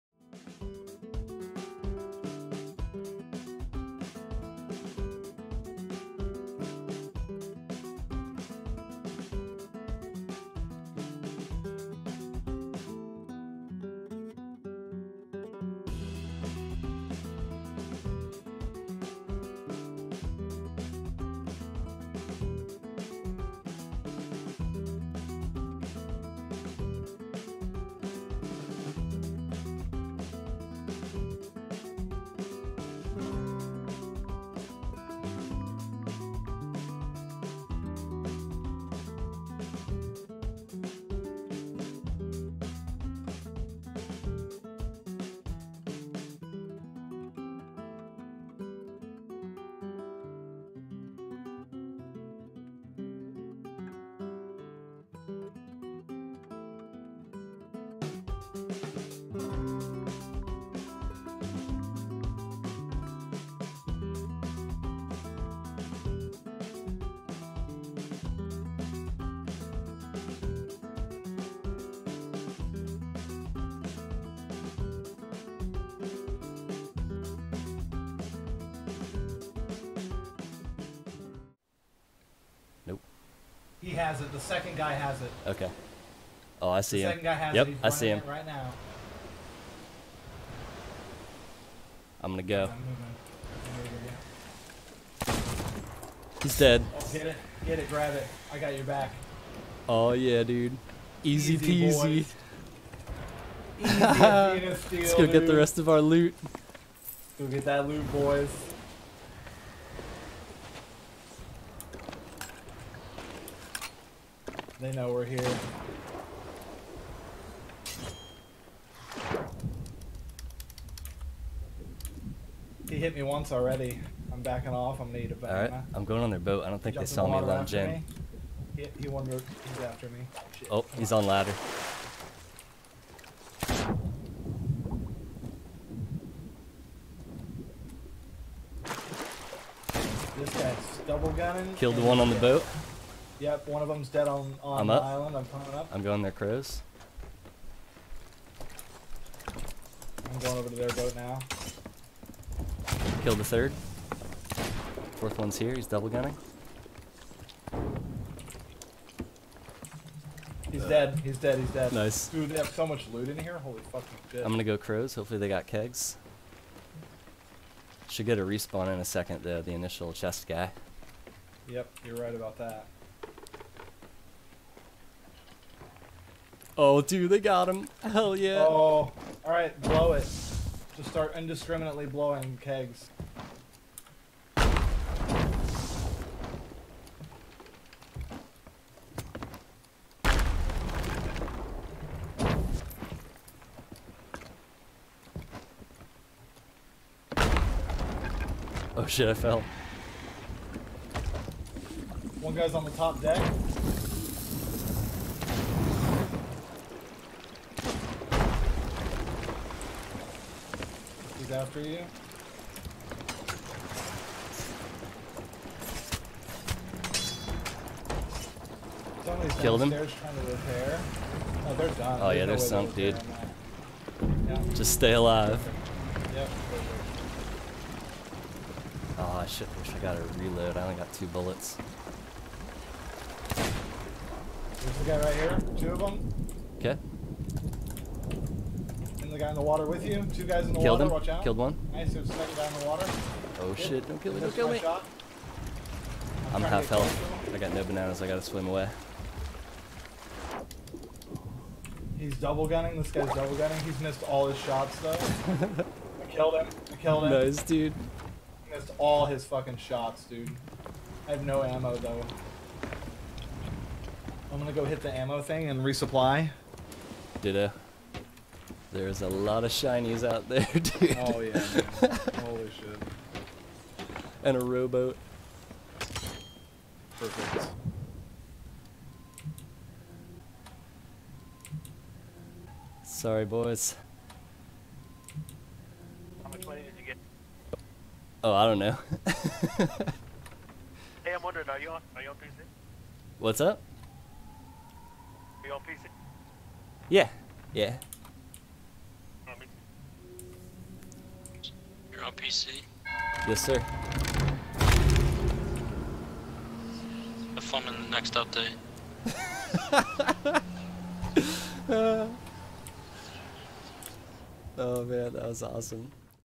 Ashen Athena Steal 1 (ft. CaptanBlueBeard, Captain Boilo)
Blue and Boilo lie patiently in wait for their prey